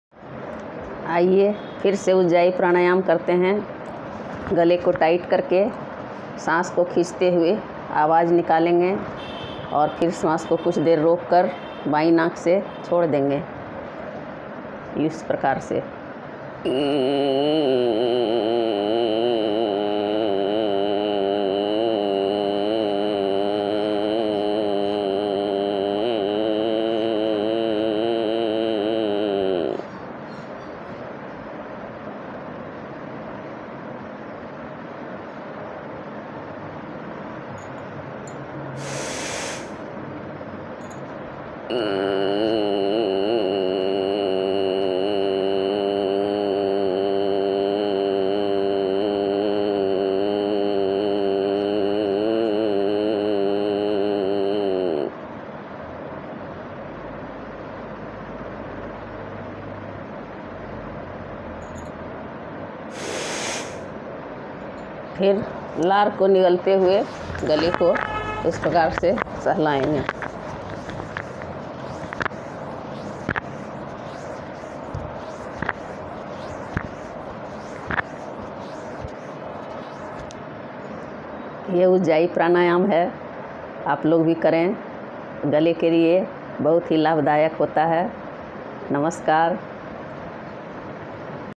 प्राणायाम उज्जायी प्राणायाम देव स्तुति
प्राणायाम।
उज्जायी प्राणायाम।
स्वस्थ रहने के लिए हमें प्राणायाम करनी चाहिए।
आज मैं आपको उज्जायी प्राणायाम करके दिखा रही हूं । जैसा कि स्वामी रामदेव जी बताते हैं।